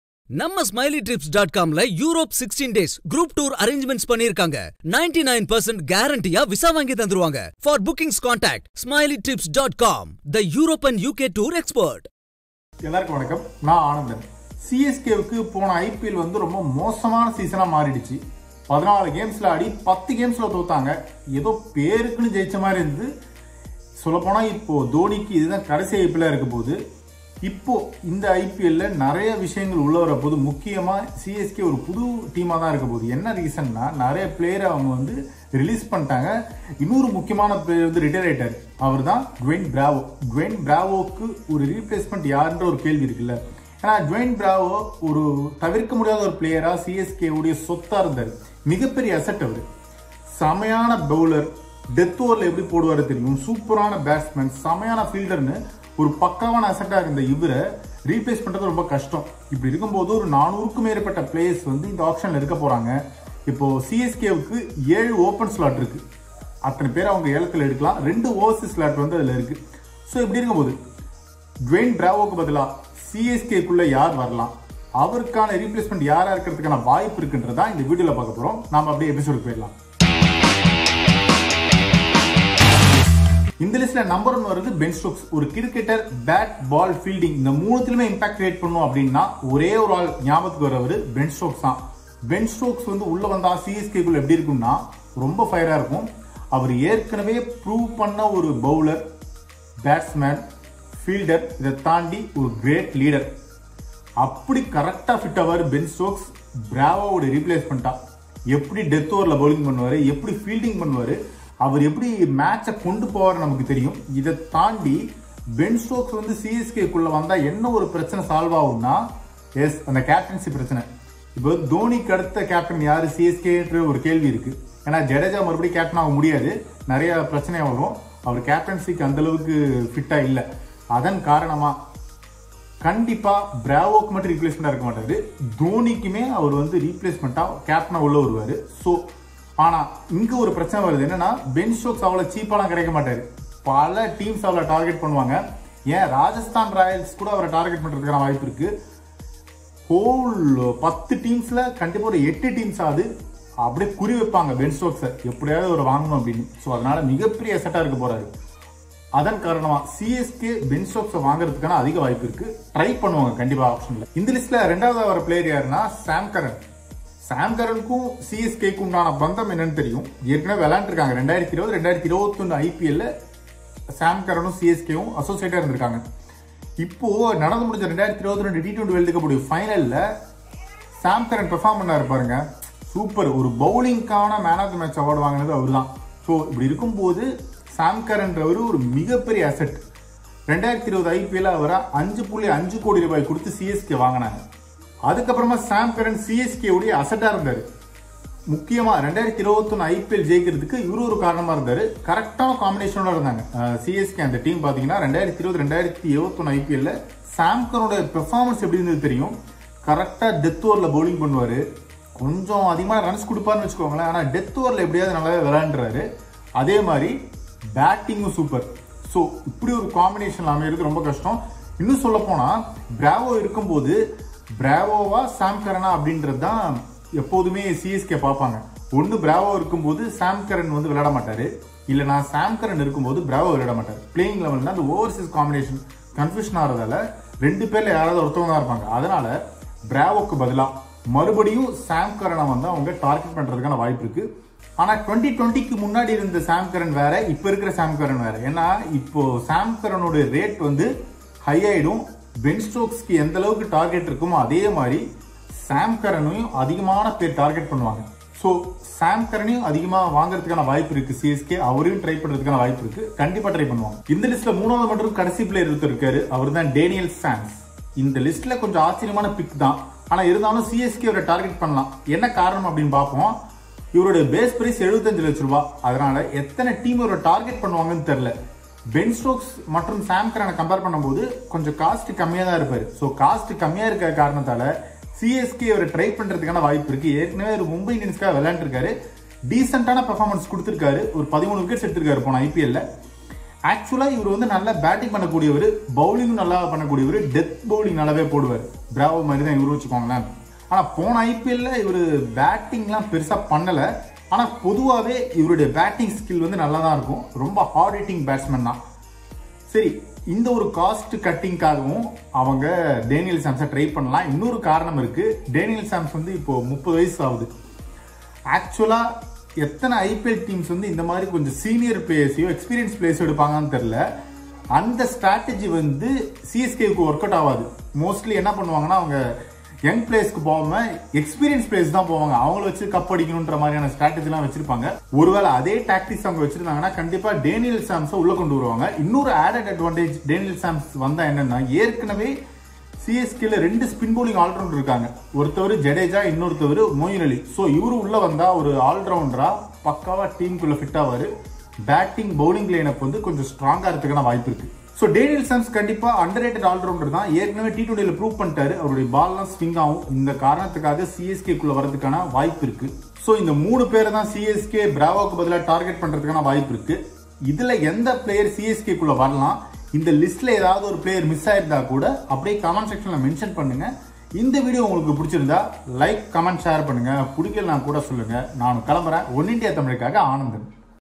IPL 2023 Auction: CSK-வில் Bravo-வுக்கு Replacement யாரு? | OneIndia Tamil
IPL 2023 Auction: Chennai Super Kings அணியில் Dwayne Bravo-வுக்கு பதிலாக வர கூடிய Players யார்